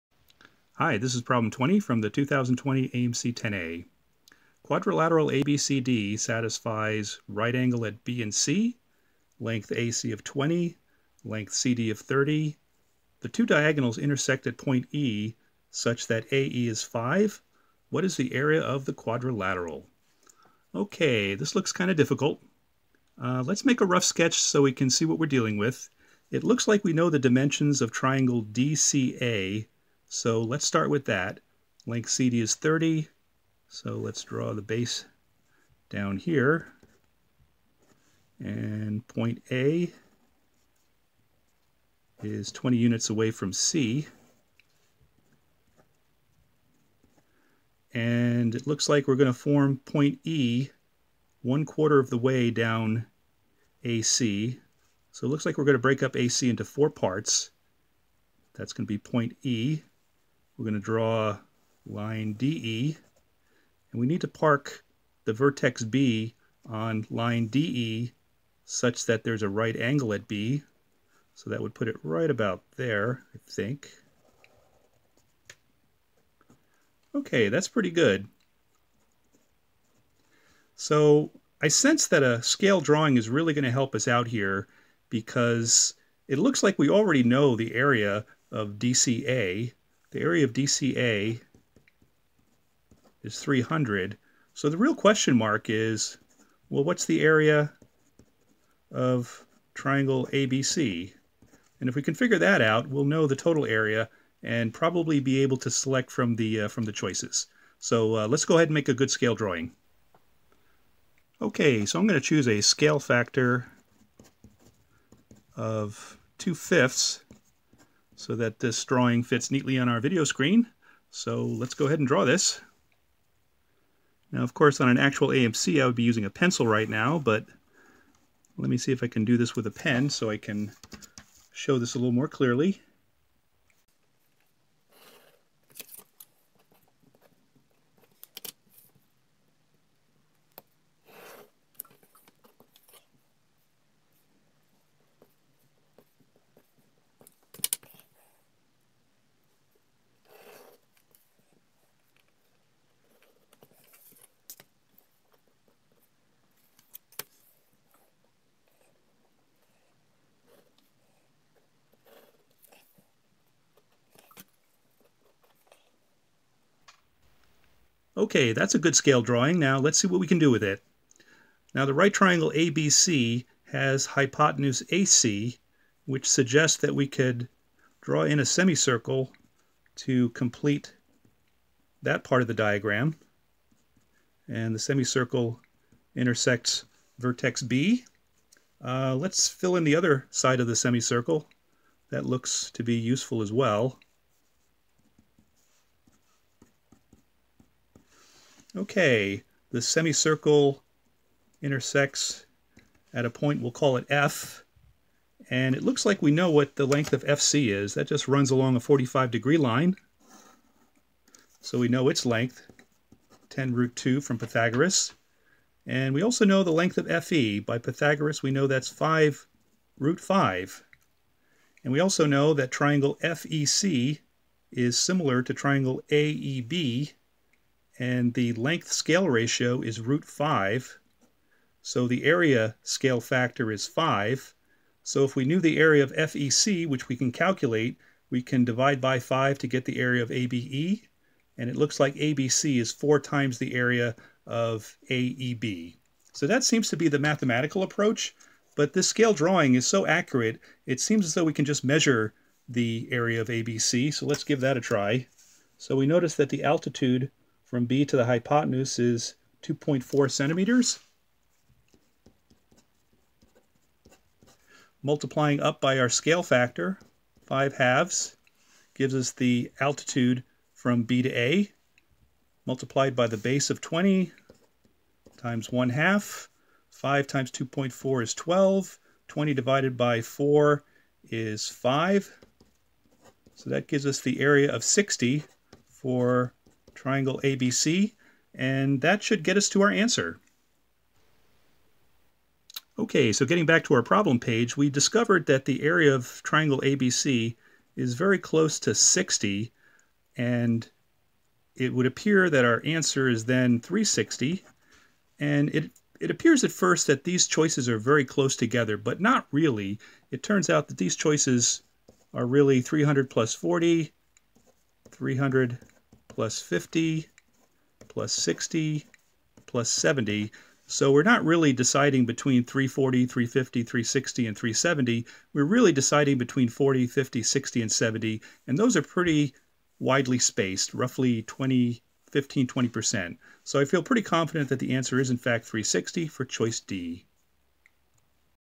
2020 AMC 10A Prob 20
🔴 IMPORTANT RULE CHANGE FOR 2021:
*Effective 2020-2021 competition cycle, only blank scratch papers, rulers, compasses, and erasers will be permitted on the AMC 10/12, AIME, and USA(J)MO.
GRAPH PAPER AND PROTRACTOR ARE NOT ALLOWED!

Tips to create and measure drawings without graph paper or protractor

🔷 Mαth Sσlution:
▲FEC ~ ▲AEB  because they are formed from two intersecting chords inside the same circle and have:
 Length Ratio, k = (5√5)/5 = √5
 Area Ratio = k² = 5
 Area[▲FEC] = ½•15•10 = 75
 Area[▲AEB] = 75/k = 75/5 = 15
 Area[▲ABC] = 4•Area[▲AEB] = 4•15 = 60

🤔 A good scale drawing helps spot ▲FEC ~ ▲AEB along with the convenient slopes of lines DE and FC.  Also offers a good "Plan B" if you can't spot them!  Coord Geometry and Similar Right Triangles are also good approaches, details below.

🔶 Choose a scale factor:
I used 2/5 but a scale factor of ½ also works well, and if you're using finer graph paper, then a 1x scale might be better.  You just want a good sized drawing with most of the critical points "on grid."

💜 Auxiliary Lines and Scale Drawing are two good topics to review in the week leading up to the AMC exam.  Good luck on the upcoming exam!

■ Coordinate Geometry Details
Place origin at the circle center:
 x² + y² = 10²
Line FB has slope=½, y-int=5:
 y = ½x + 5
Substitute into circle equation:
 x² + (½x + 5)² = 100
And solve for altitude x:
 5x²/4 + 5x - 75 = 0
 x² + 4x - 60 = 0
 (x + 10)(x - 6) = 0
Altitude, x = 6
Area[▲ABC] = ½•20•6 = 60

■ Similar Right Triangle Details
Draw BG, the altitude from B to AC
Label  x = EG → AG = (5 - x)
▲DCE ~ ▲BGE  → BG = 2x
BG² = AG • GC
 (2x)² = (5 - x)(15 + x)
 4x² = 75 - 10x - x²
 x² + 2x - 15 = 0
 (x - 3)(x + 5) = 0 → x = 3
Altitude, BG = 2x = 6
Area[▲ABC] = ½•20•6 = 60